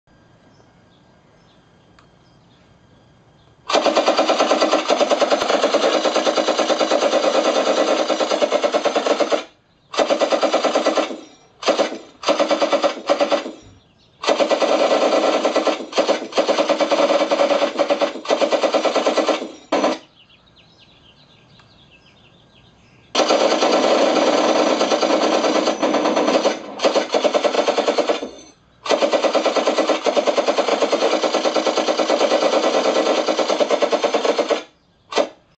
Parkzone T-28 RC plane - machine gun sounds w/ lights
Custom circuit to play out machine gun sounds and lights.  Loud 20W amp.  All sounds controlled from the radio.